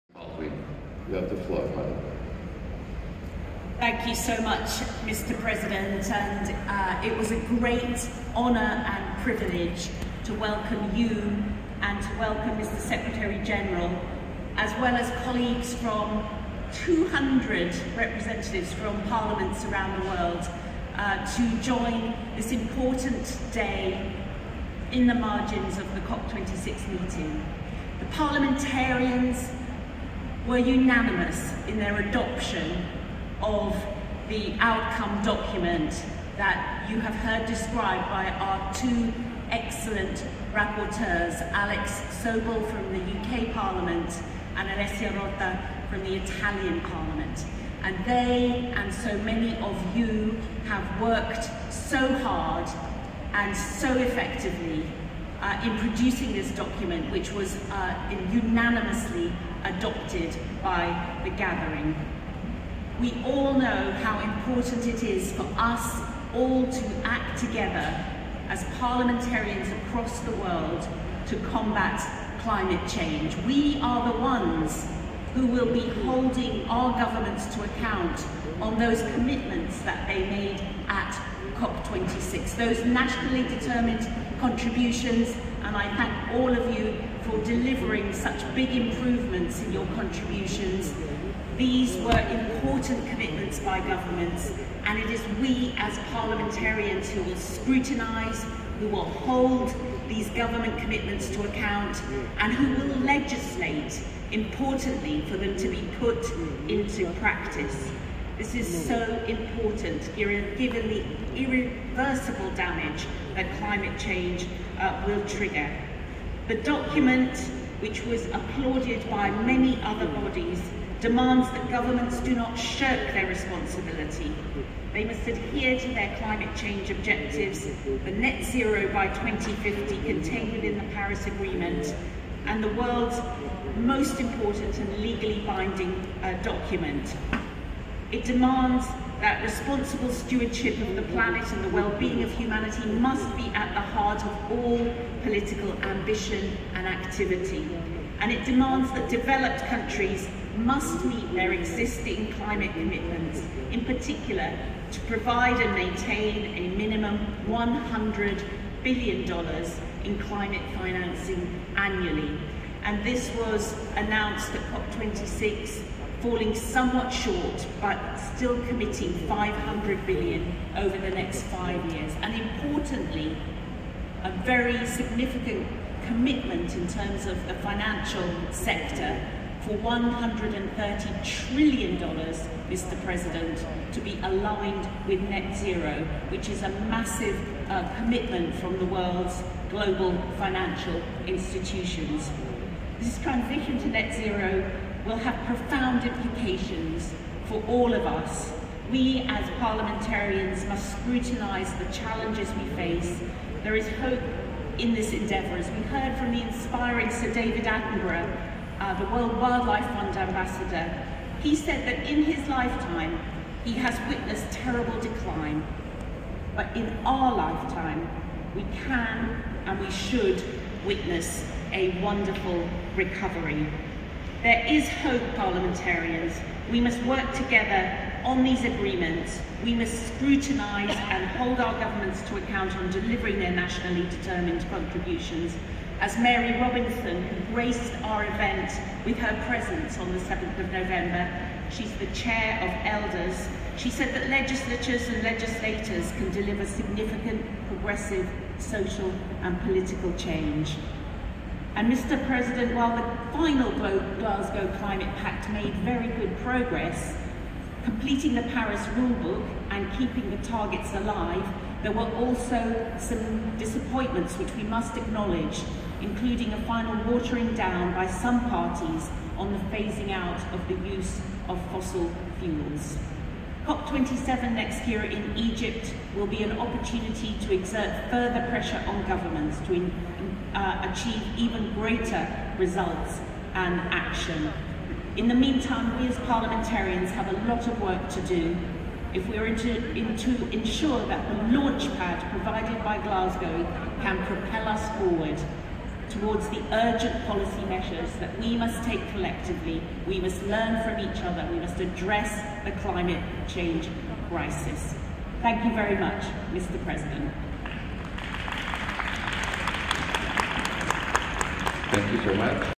BGIPU Chair reports to the 143rd IPU Assembly on the COP26 Parliamentary meeting in Glasgow.
With some 200 parliamentarians, officials and experts participating, the world's parliaments committed to taking urgent action on climate change in Glasgow, United Kingdom at the UN Climate Change Conference, COP26. The parliamentarians were gathered in Glasgow on 7 November at a parliamentary meeting co-organized by the IPU and BGIPU.  As reported to the 143rd IPU Assembly in Madrid on 30 November by BGIPU Chair, Harriett Baldwin MP, in their final adopted outcome document MPs laid out an ambitious roadmap for increased parliamentary engagement to address climate change. They underlined the importance of parliaments, with their core legislative, oversight, budgetary and representation functions, in ensuring the implementation of the climate change objectives contained in the Paris Agreement, the world’s major legally-binding international treaty on climate change.